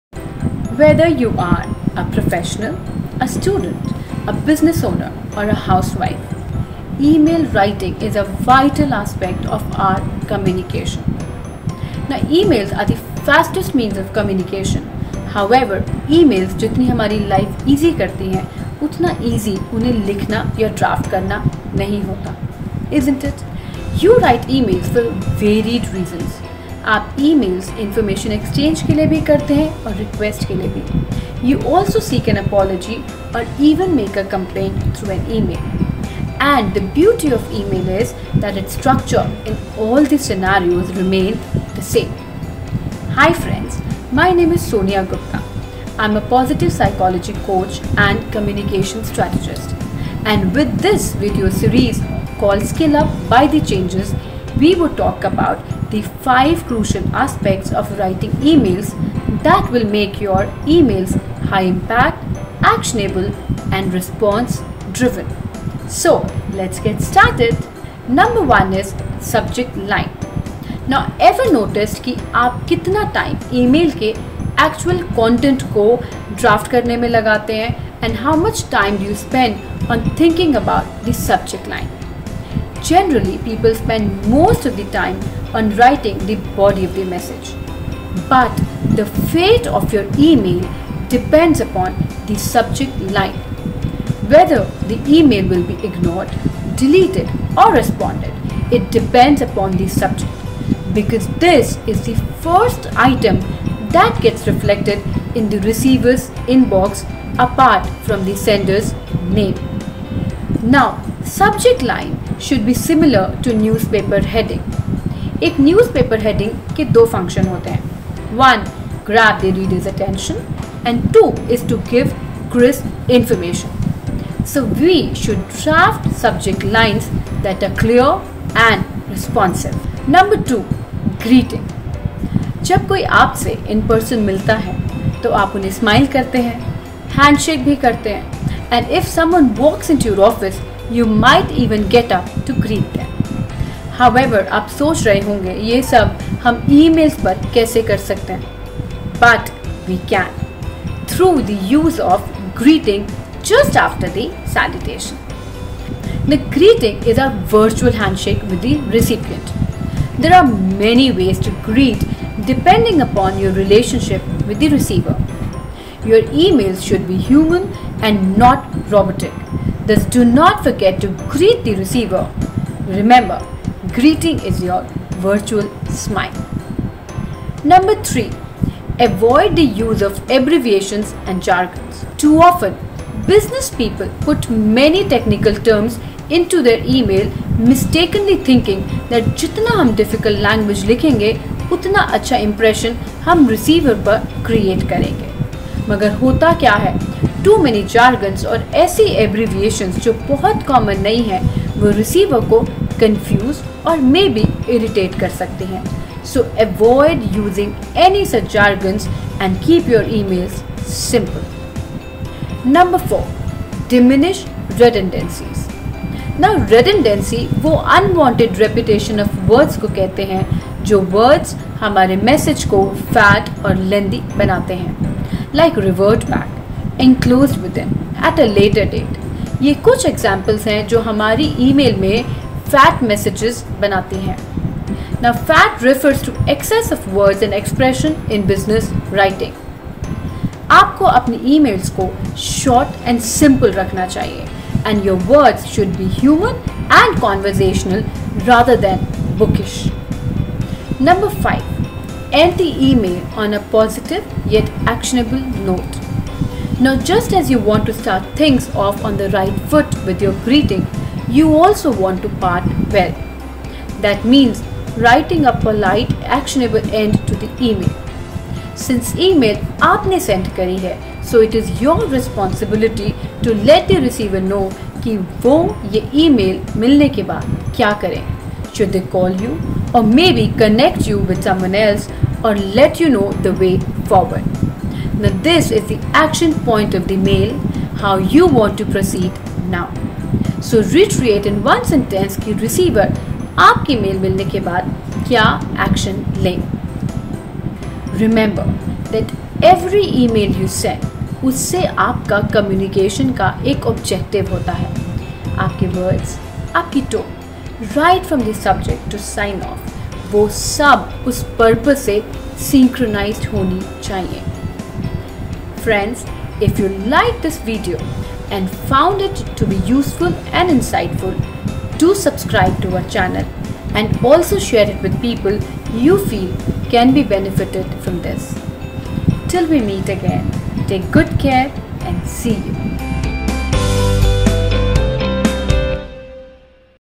5 Tips On How To Improve Email Writing Skills | Professional Email Kaise Likhe | The Changes Academy
5 Tips On How To Improve Email Writing Skills | Professional Email Kaise Likhe
This video is about how write professional emails and how to improve your email writing skills
learn professional email writing skills professional email writing format

Anatomy of High Impact Emails
Every email you write has the same basic structure: Subject line, greeting, email body, and closing. But as with every written form of professional communication, there’s a right way to do it and standards that should be followed. This video series, Skill Up by The Changes brings to you some key elements of High Impact Emails.

The speaker, Sonia Gupta is the founder & master trainer at The Changes and she has trained more than 30k professional pan India.